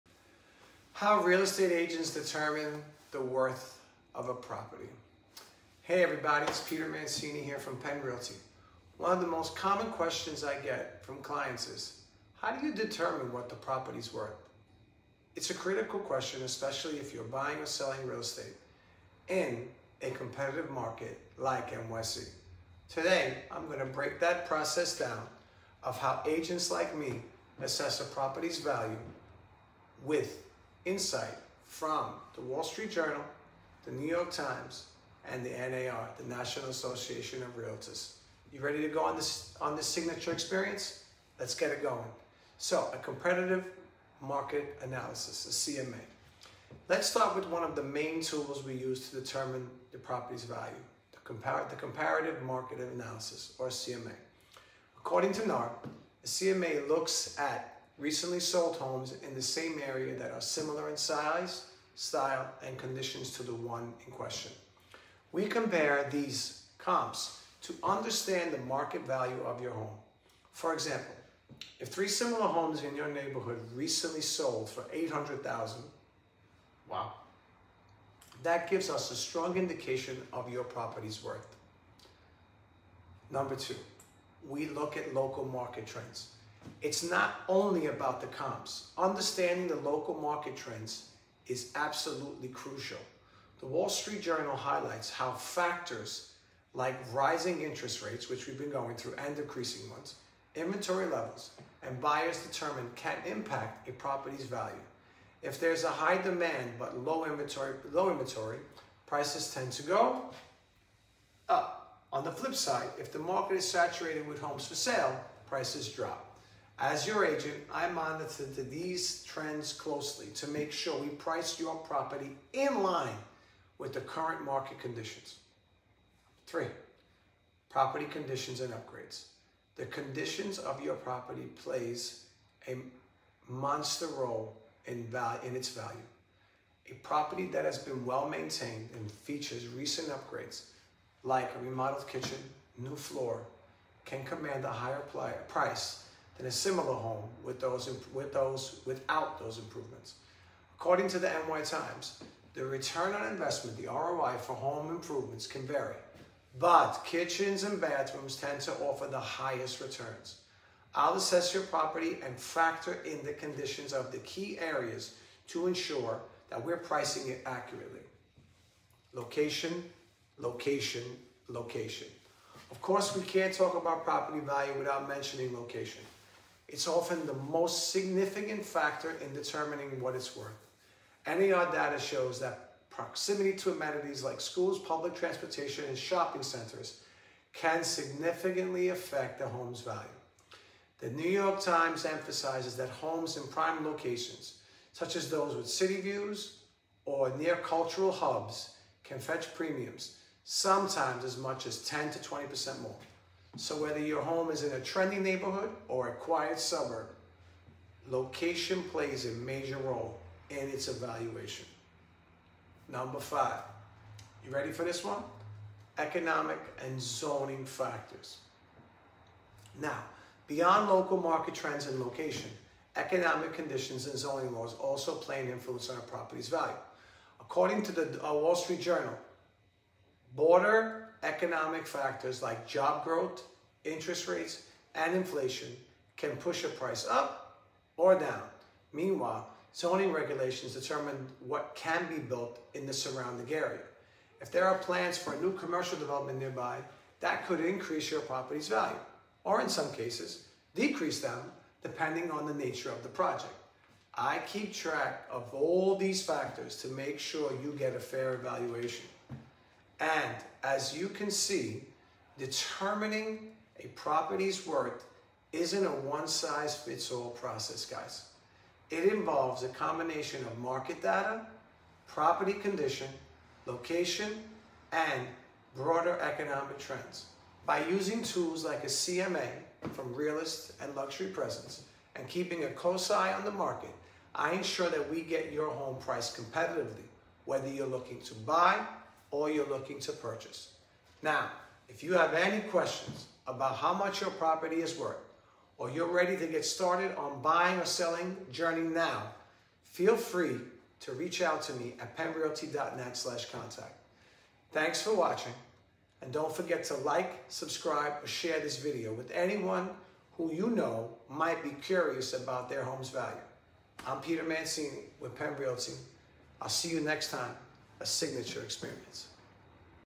Understanding Property Valuation 🔑🏡
Hey, it's Peter Mancini from PenRealty.net! I'm breaking down how we determine the worth of a property using market analysis, trends, and location insights. Join me to learn more! RealEstate PropertyValue NYC 🏙️💰